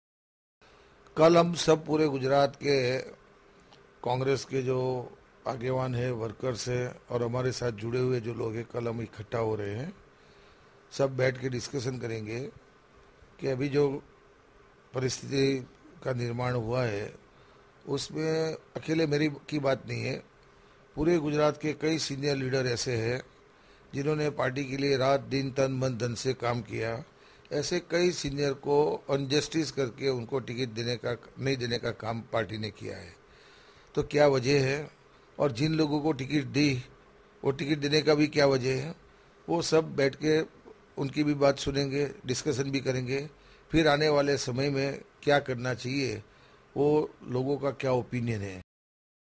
Congress Meet at Ahmedabad, Pre-election Discussion
Congress members denied tickets in the upcoming polls have decided to meet in Ahmedabad on Sunday to chalk out their next course of action. Former Congress leader Narhari Amin has confirmed that the meeting will take place and that those having issues with the way the Congress is conducting itself in Gujarat will come up for discussion.